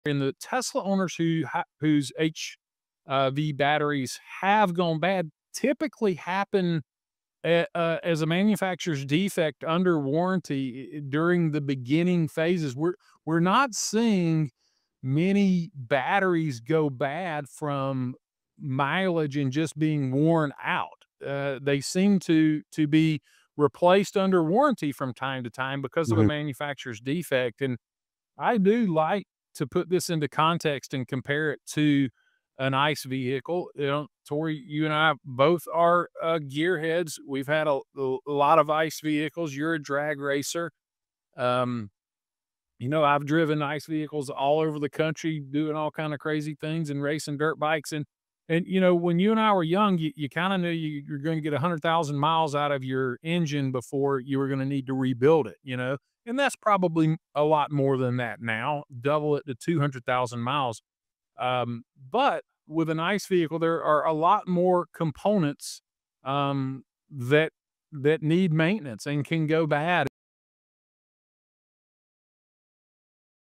Tesla HV Battery Reliability: Why Failure Is Rare
Tesla battery failures are often due to manufacturer defects, covered under warranty. Unlike ICE vehicles, mileage isn't the main factor. More complex maintenance in ICE vehicles.